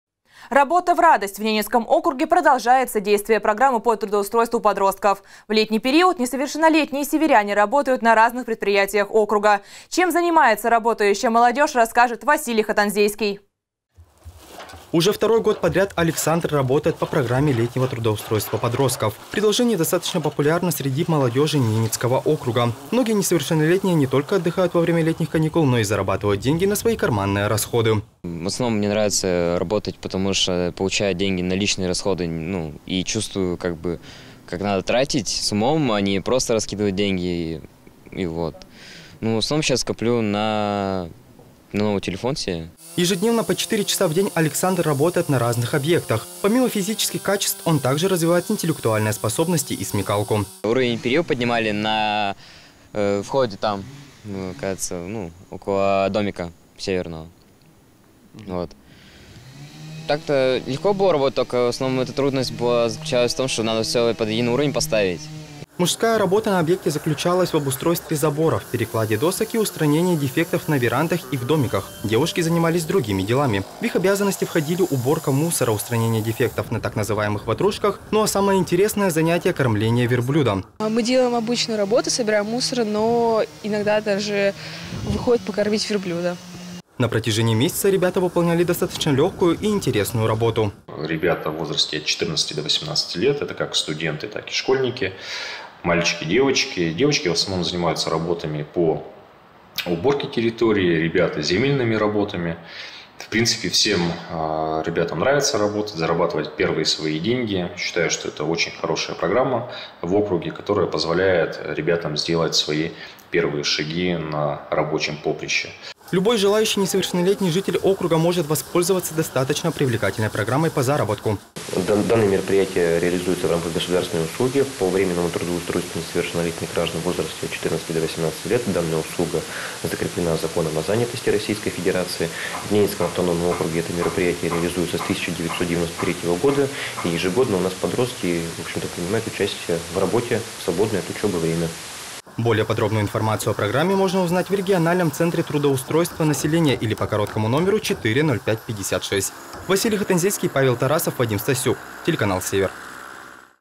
Трудоустройство подростков
Работа в радость. В Ненецком округе продолжается действие программы по трудоустройству подростков. В летний период несовершеннолетние северяне работают на разных предприятиях округа.